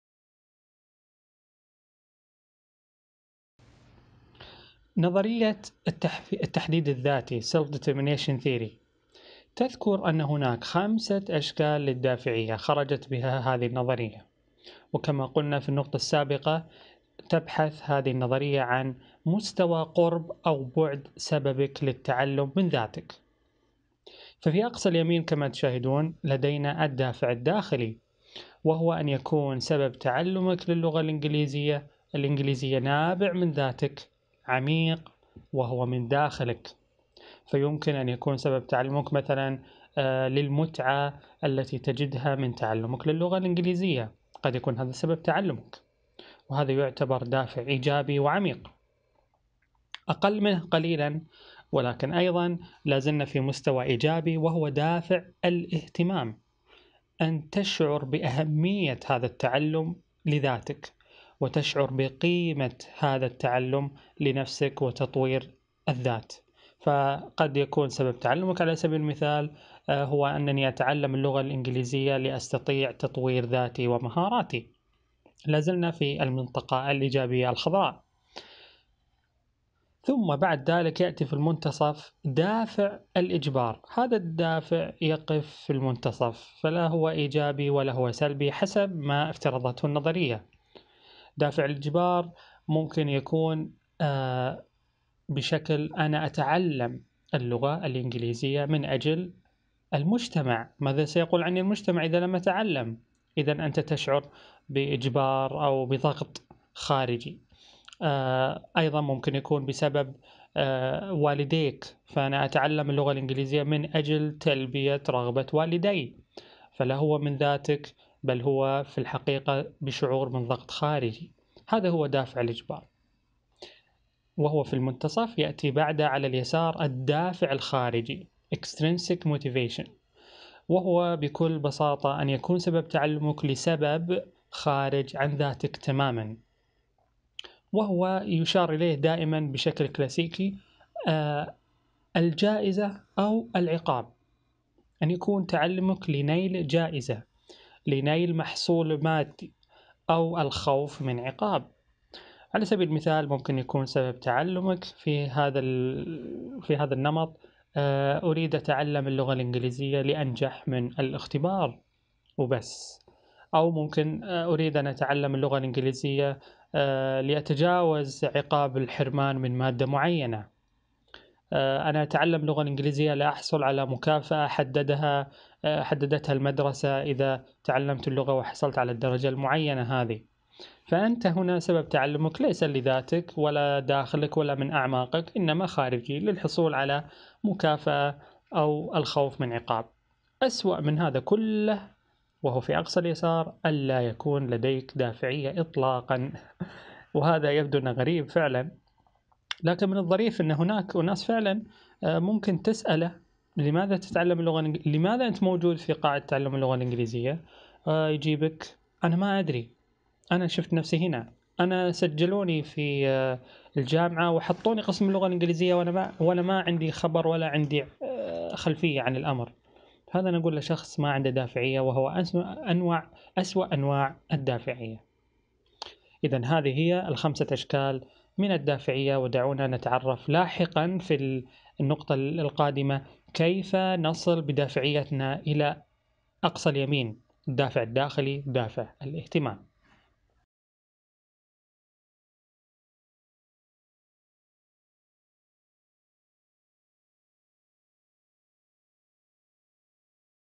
رواق: كيف تحفز نفسك بفاعلية نحو تعلم اللغة الانجليزية- 3 - 3
رواق: كيف تحفز نفسك بفاعلية نحو تعلم اللغة الانجليزية، المحاضرة الثالثة، الجزء الثالث: الأشكال الخمسة